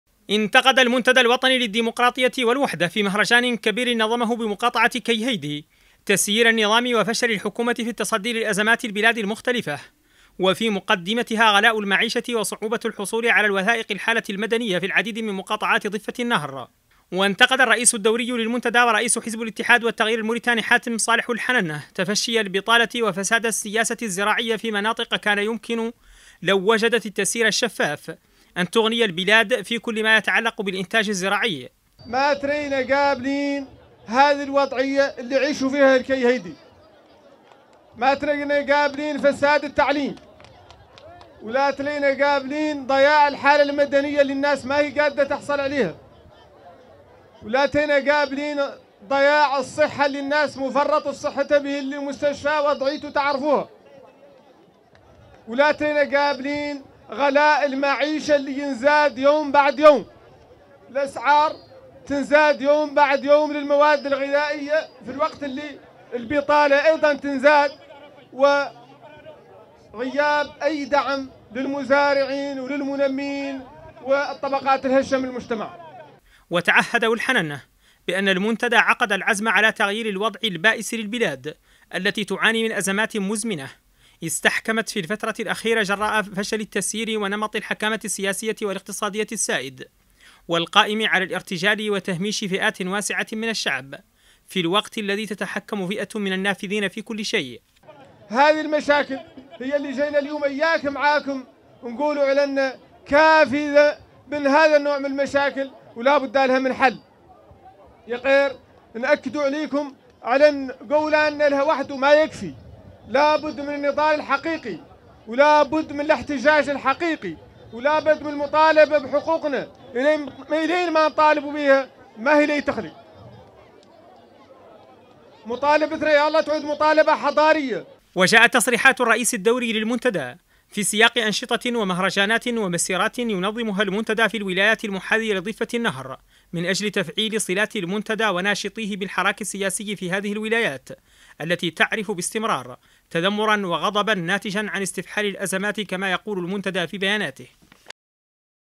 المنتدى يرفض ما سماه فساد التعليم وضياع الحالة المدنية بكيهيدي تقرير الحافظ ولد الغابد.
قال رئيس المنتدى الوطني للديمقراطية والوحدة صالح ولد حننا إن المنتدى يرفض ما سماه فساد التعليم، وضياع الحالة المدنية، وغلاء المعيشة في مدينة كيهيدي.
ودعا ولد حننا خلال مهرجان نظمه المنتدى بالمدينة إلى النضال السلمي من أجل القضاء على ما وصفه التهميش والغبن.